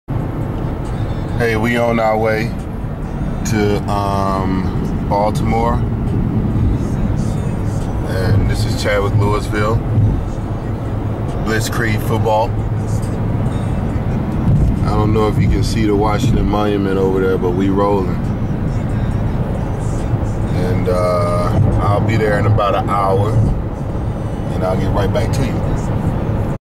See You in Baltimore: Road trip to Towson University Baltimore, Maryland Johnny Unitas Stadium
On my way to a football event at Towson, University. Started out driving through Washington, DC